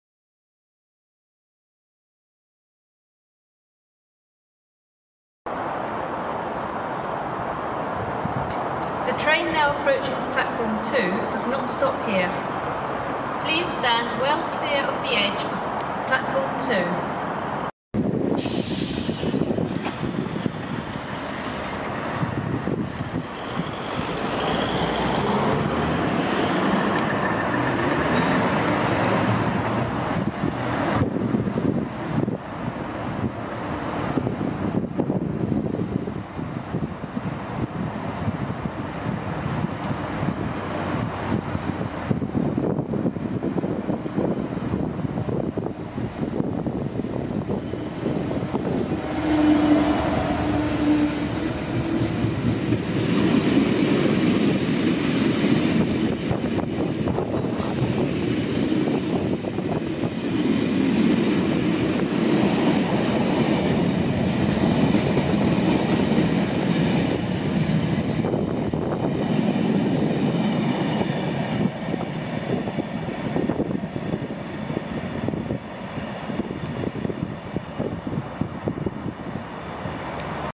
Freight Train At Waltham Cross 16 December 2015
The 02:03 Moreton-on-Lugg to Harlow Mill GBRf freight train passes through Waltham Cross at 12:00 on Wednesday 16 December 2015. Sorry the video is a bit shaky; also I was pointing into the Sun.